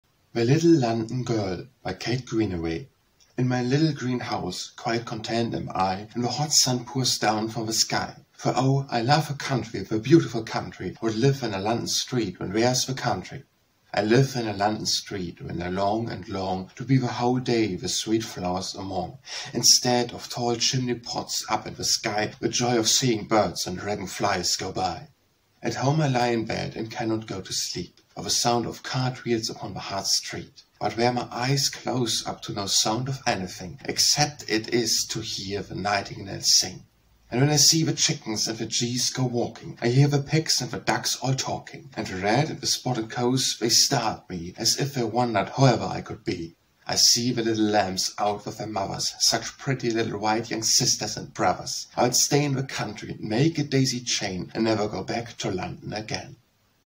The Little London Girl - Kate Greenaway
The Little London Girl
Kate Greenaway

In my little Green House, quite content am I,
When the hot sun pours down from the sky;
For oh, I love the country the beautiful country.
Who'd live in a London street when there's the country?

I live in a London street, then I long and long
To be the whole day the sweet Flowers among
Instead of tall chimney-pots up in the sky,
The joy of seeing Birds and Dragon Flies go by.

At home I lie in bed, and cannot go to sleep,
For the sound of cart-wheels upon the hard street.
But here my eyes close up to no sound of anything
Except it is to hear the nightingales sing.

And then I see the Chickens and the Geese go walking,
I hear the Pigs and the Ducks all talking.
And the Red and the Spotted Cows they stare at me,
As if they wondered whoever I could be.

I see the little Lambs out with their mothers
Such pretty little white young sisters and brothers.
Oh, I'll stay in the country, and make a daisy chain,
And never go back to London again.